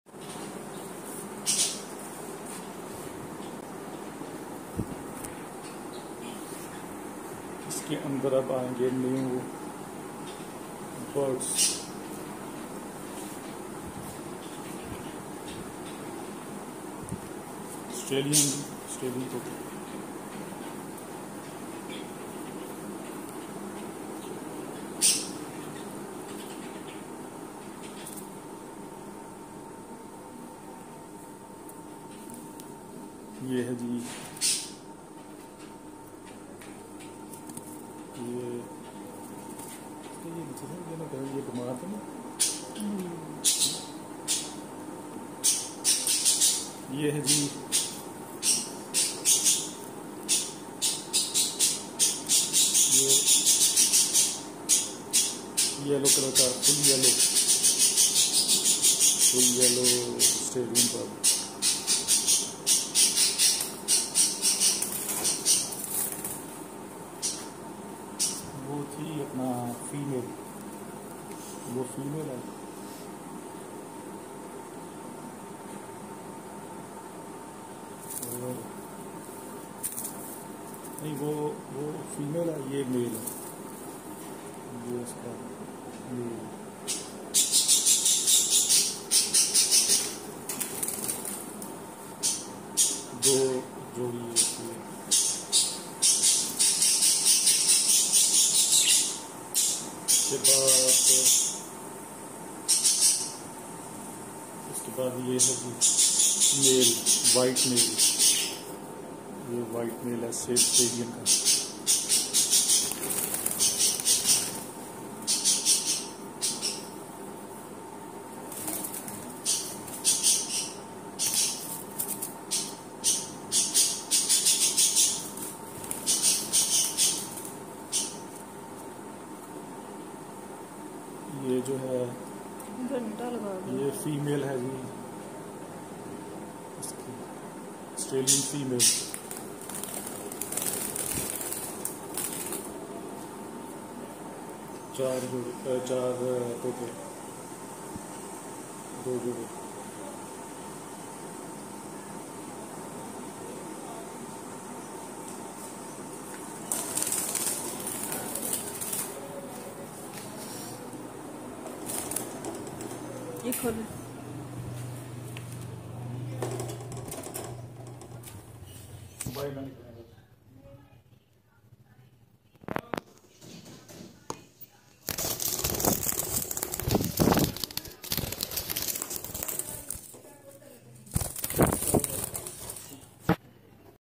New Pets | 2 pair of 🦜 | Australian Parrots in Pakistan | Budgies
New Pets, 2 pair of Australian Parrots 🦜, Australian Parrots in Pakistan, Budgies @haseebezad

I just bring 2 pair of Australian parrots. One is white and the second is yellow.